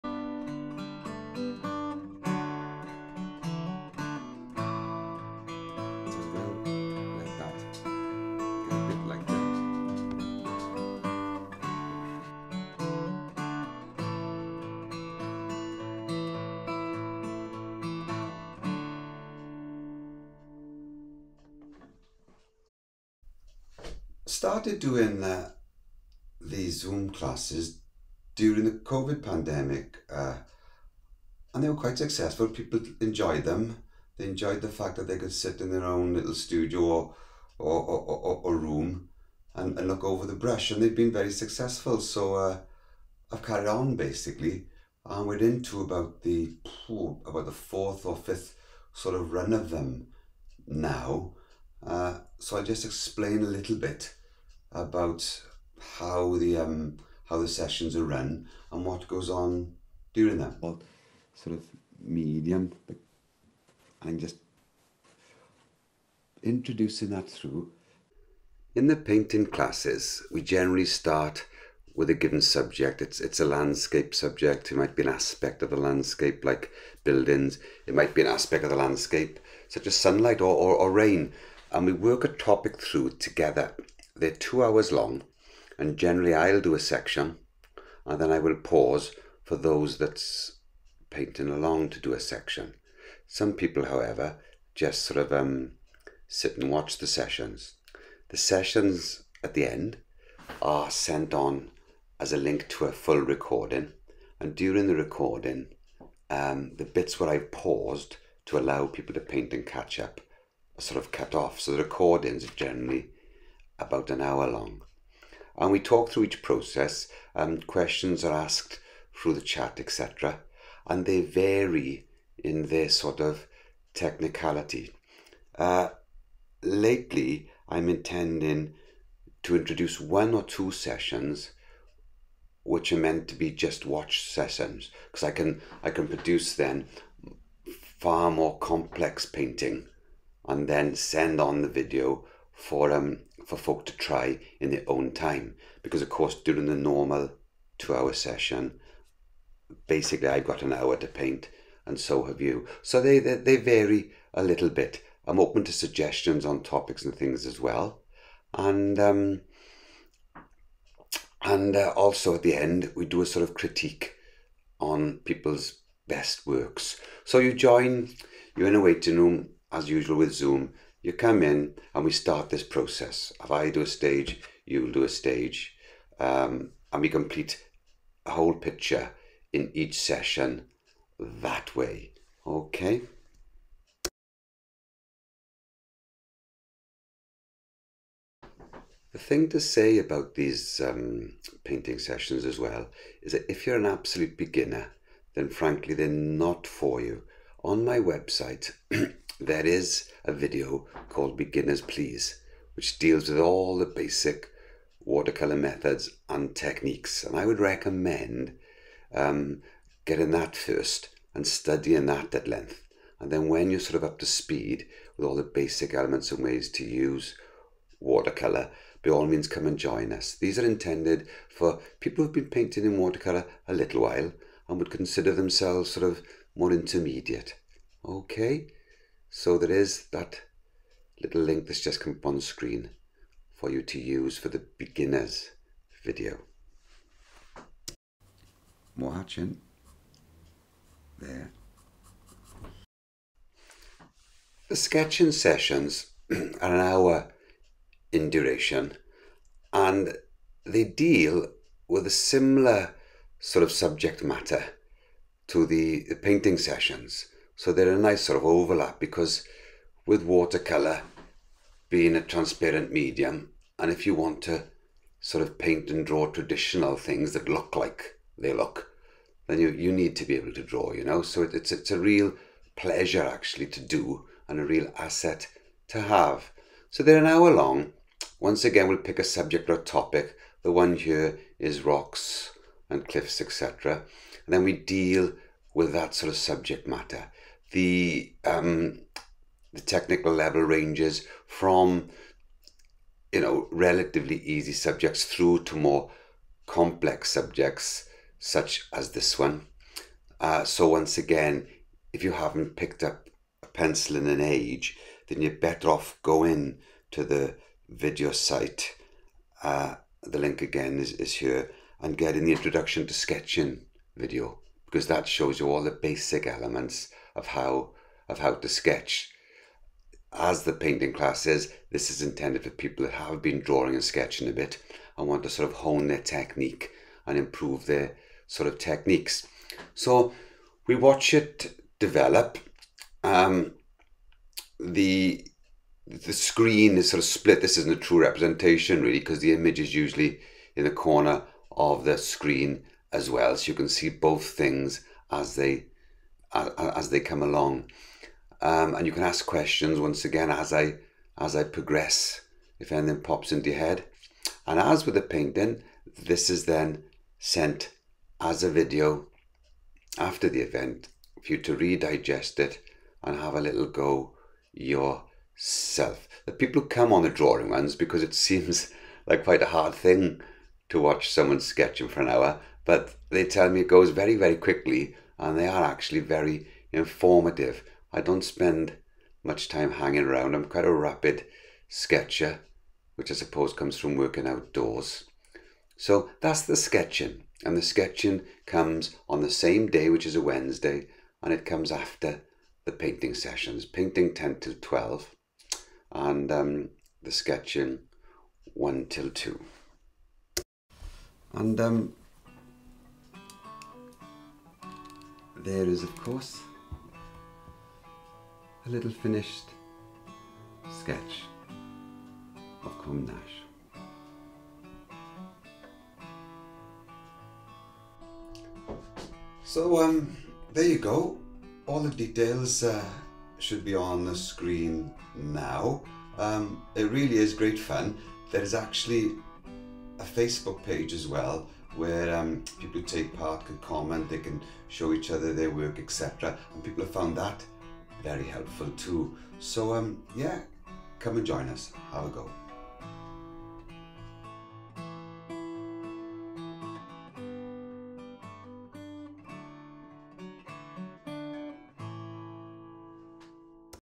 An introduction to zoom classes by Peter Cronin rsma SD 480p
Here is a brief introduction to the format of my zoom classes.